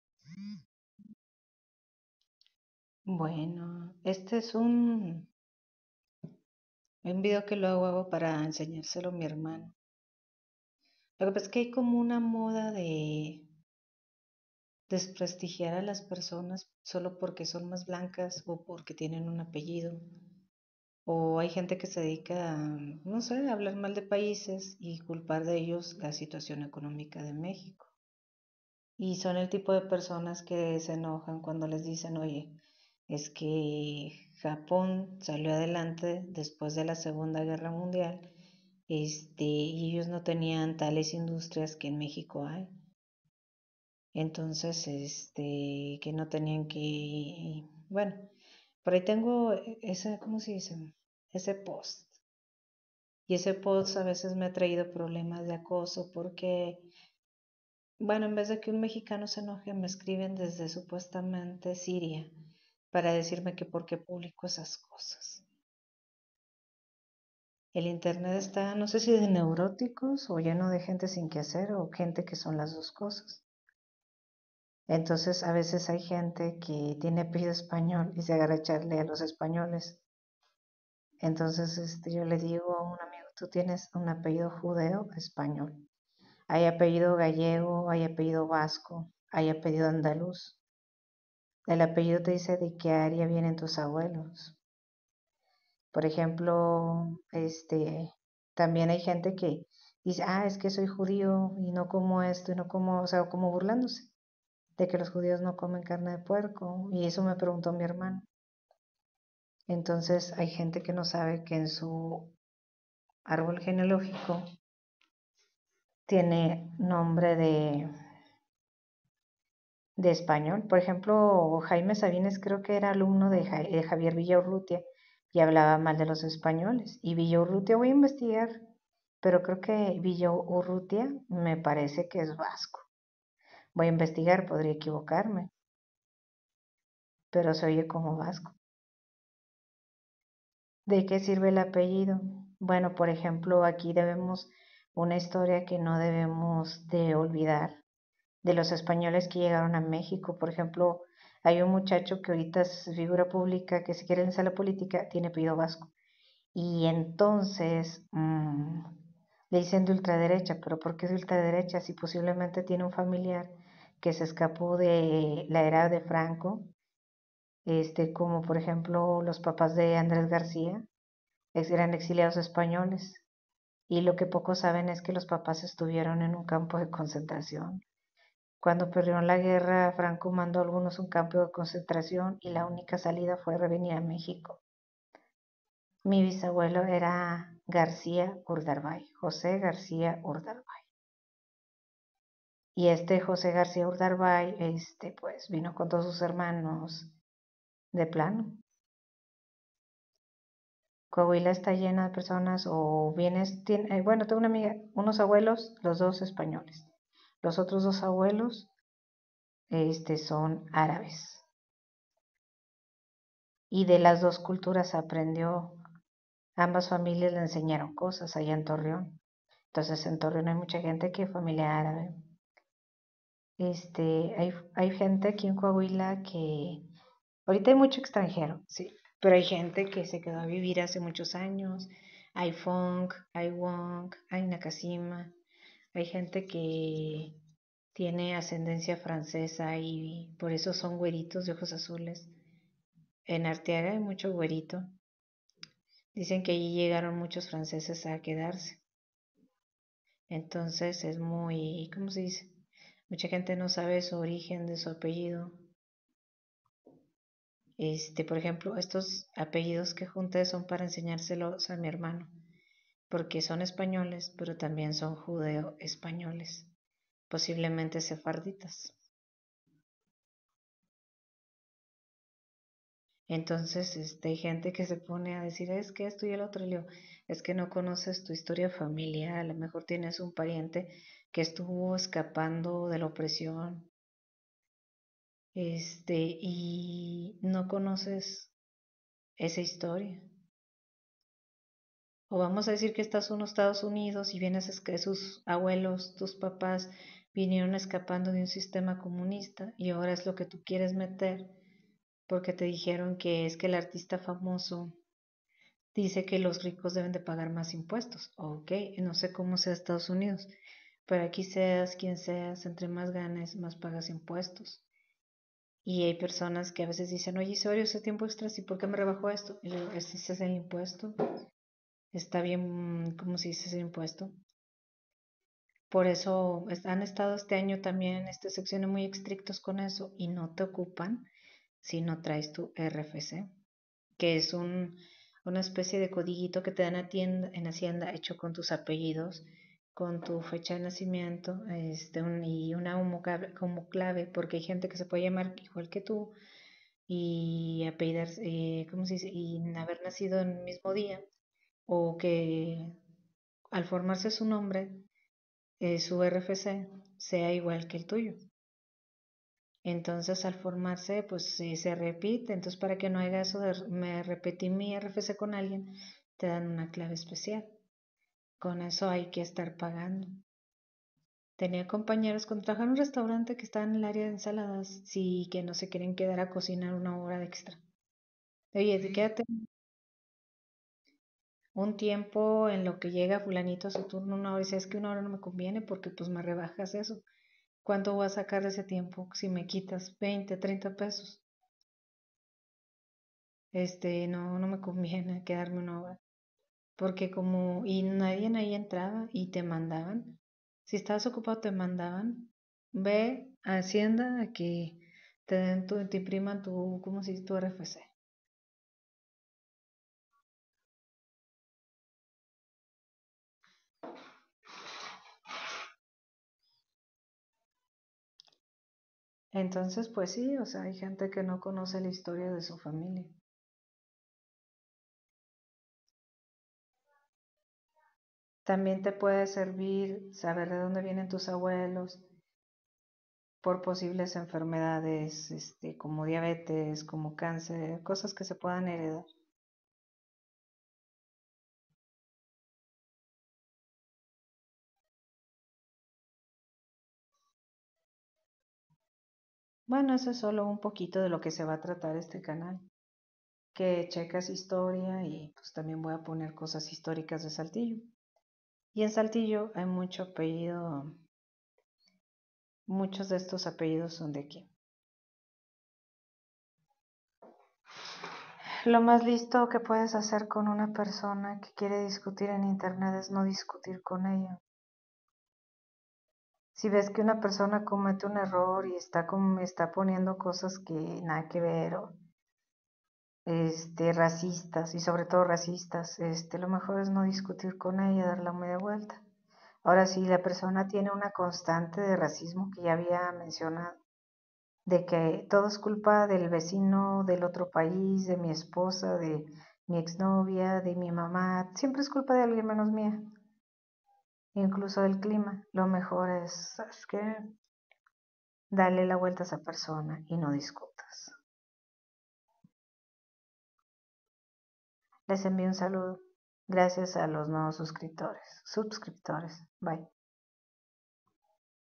apellidos.
tienes que conocer tu historia familiar quizás tu abuelo o bisabuelo escapó de una dictadura.